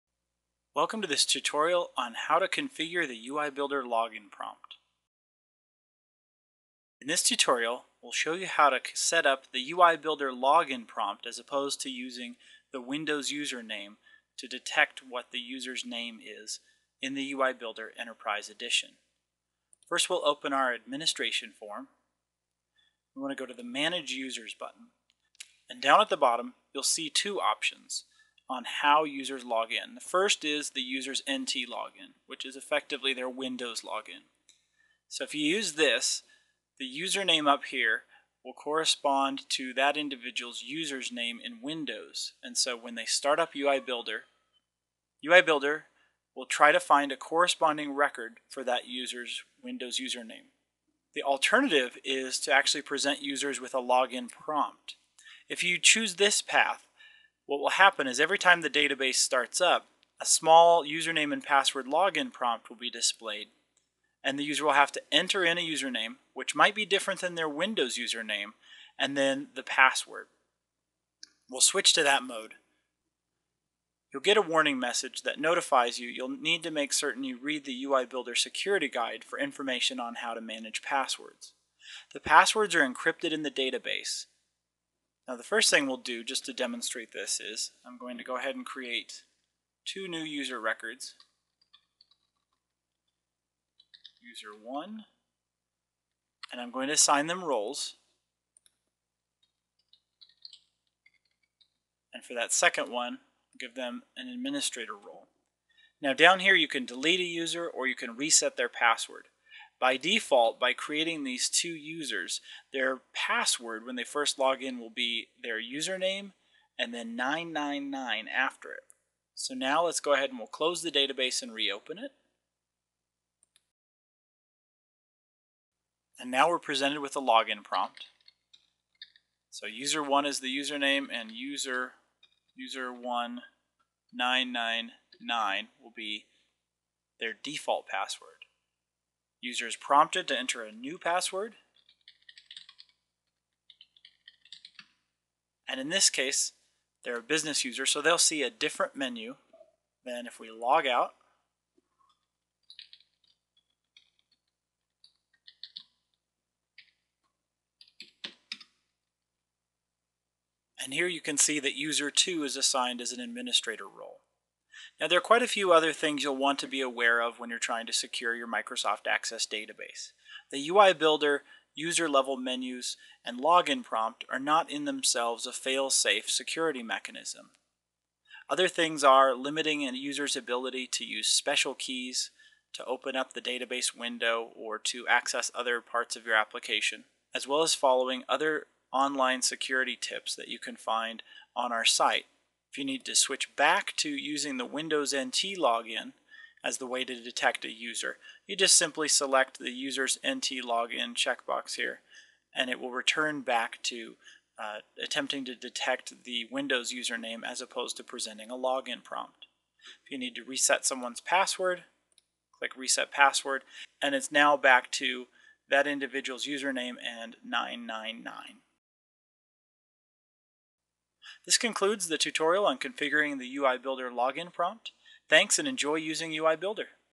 Configuring the UI Builder for Access Login Prompt
This tutorial demonstrates how to set up the user login prompt included with UI Builder for Microsoft Access - Enterprise Edition.